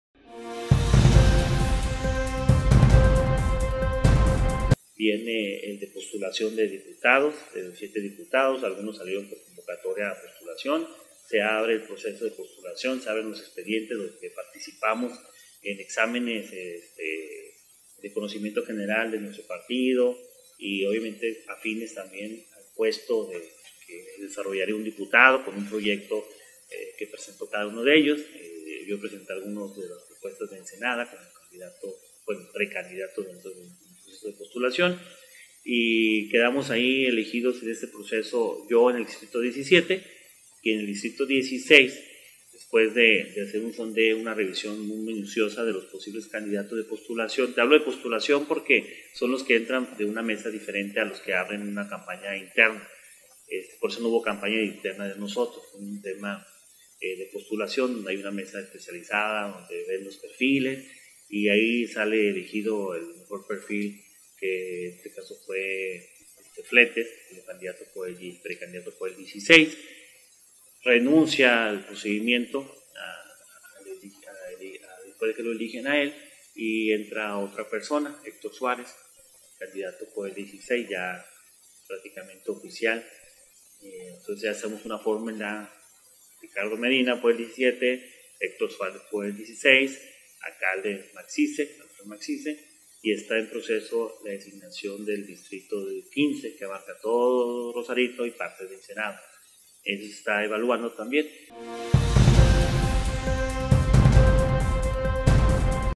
Nota :: Lic. Ricardo Medina Fierro :: NOTICIERO TREINTA33.TV
En atención a los protocolos y apegándose a las leyes electorales, el Lic. Ricardo Medina Fierro, se separa formalmente de su cargo para postulación de elección popular directa.

Noticiero TREINTA33.TV con Oxibellis Ruíz.